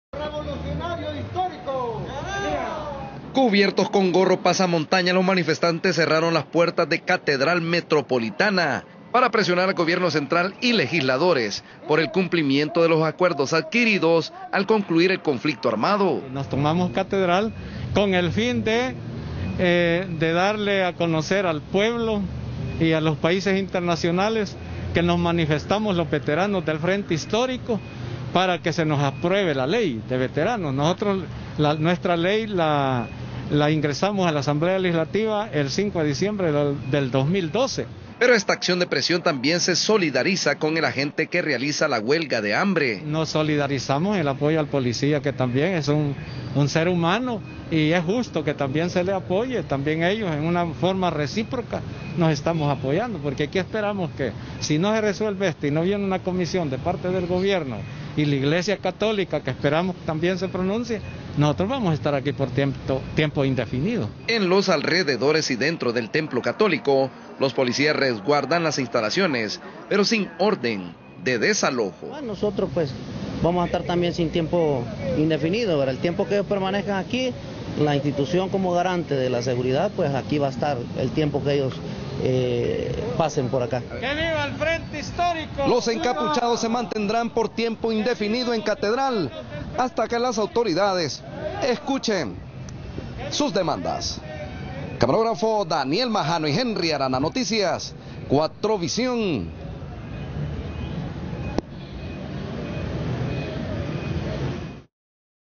Protesta en Catedral Metropolitana.
Veteranos del Frente Histórico se tomaron Catedral Metropolitana como medida de presión, entre otras exigencias, en apoyo a un agente de la PNC en huelga de hambre. (22/05/2014).